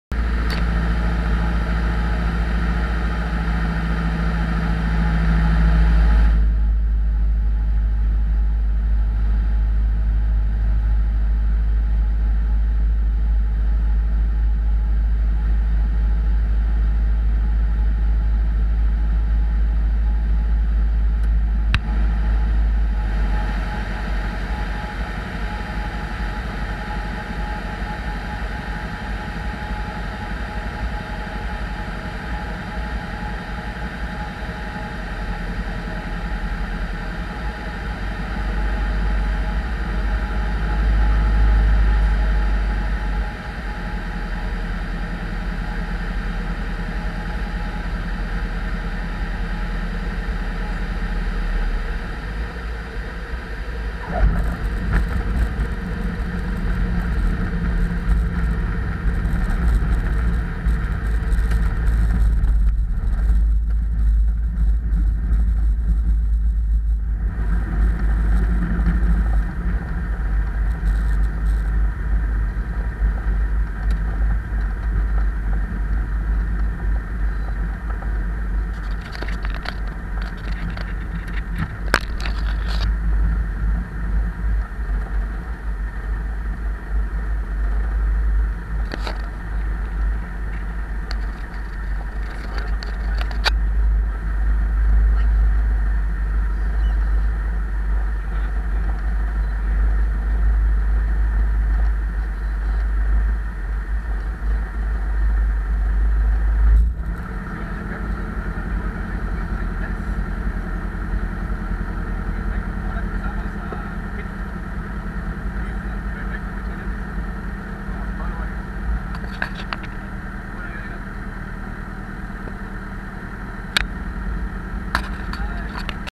LANDING PUERTO JIMENEZ COSTA RICA
Landing Puerto Jimenez,
MRPJ - Puerto Jimenez, Costa Rica - Peninsula De Osa
Capitan: Augusto James Lawrence
FULL HD